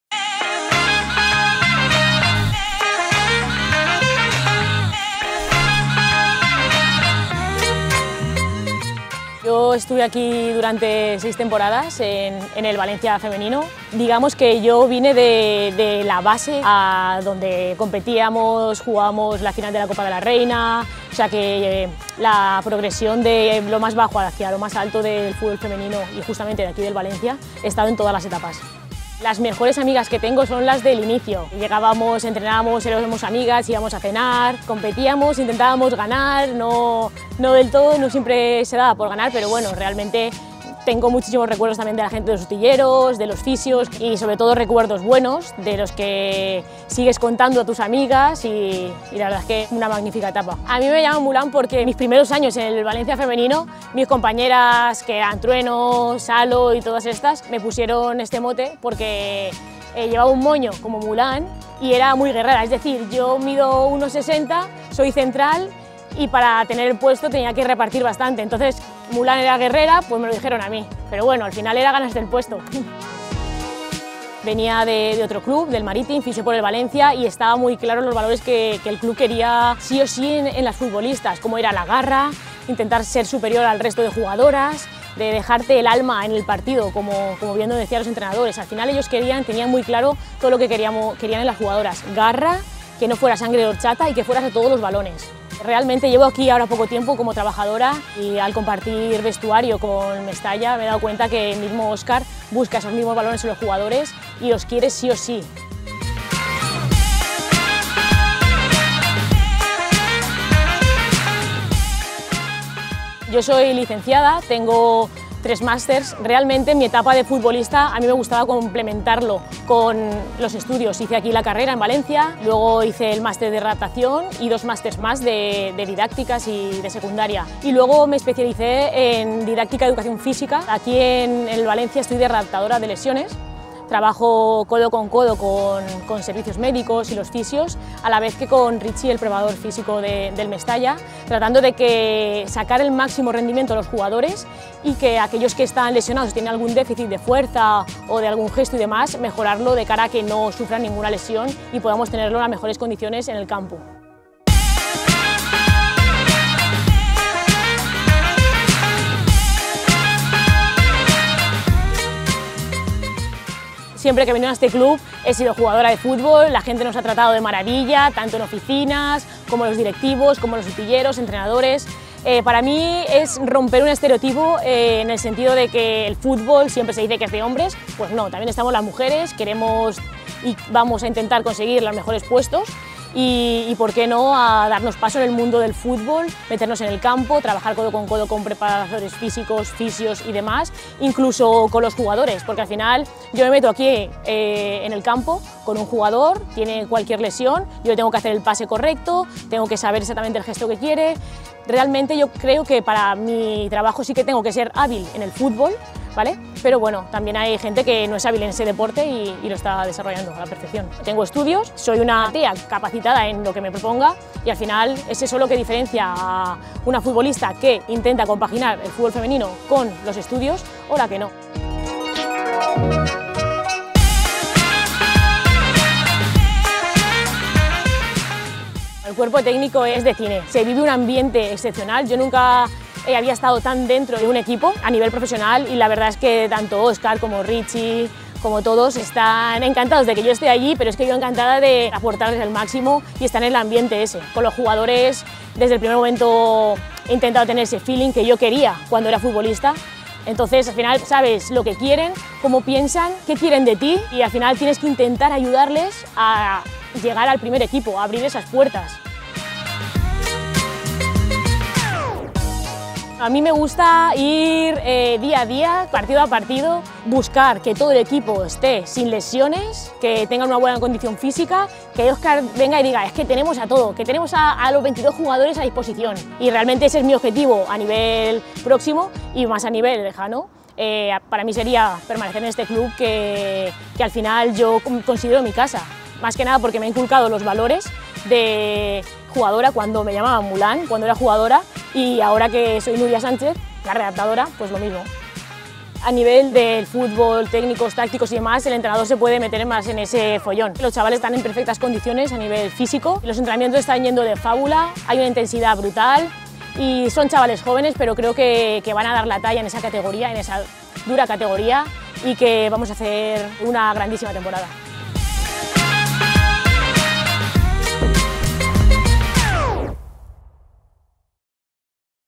💪🦇UNA “GUERRERA” PARA RECUPERARLOS A TODOS
La exjugadora del VCF Femenino Nuria Sánchez, vuelve al Valencia CF ahora como readaptadora en el VCF Mestalla.

Former VCFF player Nuría Sánchez returns as part of the VCF Mestalla backroom staff as strength and conditioning coach.